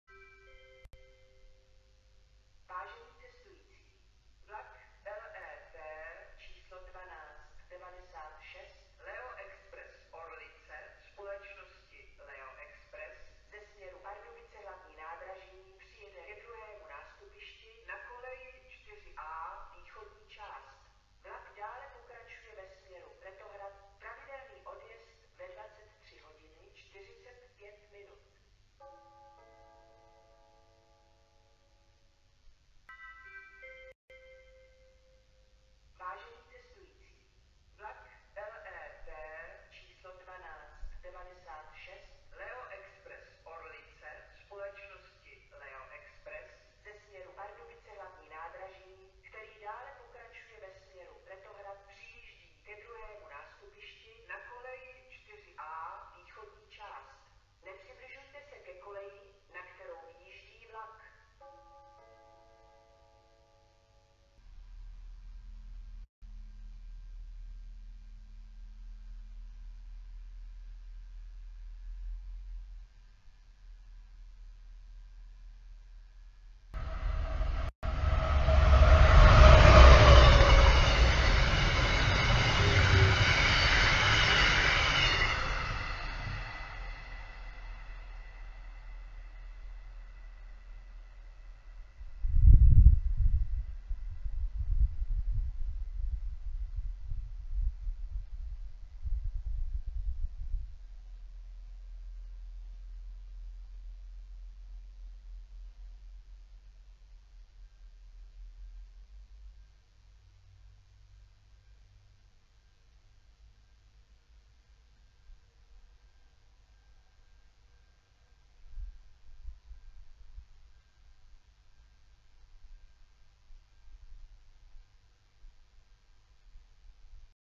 Staniční hlášení a vlak Ústí nad Orlicí - LET 1296 Leo Express Orlice + Průjezd Arriv, 17.1.2021
0:00 LET 1296 Leo Express Orlice ( přijede )
0:32 LET 1296 Leo Express Orlice ( přijíždí )
1:04 Příjezd vlaku LET 1296 Leo Express Orlice
1:21 Průjezd vlaků Arriva + Odjezd vlaku LET 1296 Leo Express Orlice